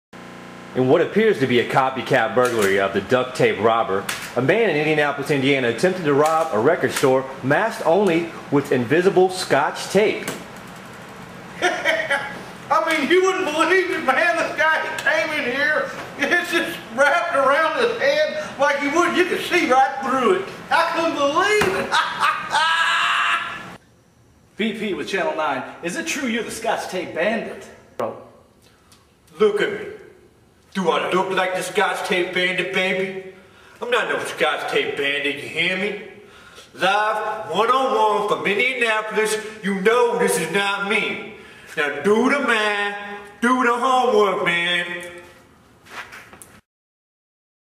duct tape bandit
Here WKIDD channel 9 reports on a possible copycat robber who uses scotchtape as a guise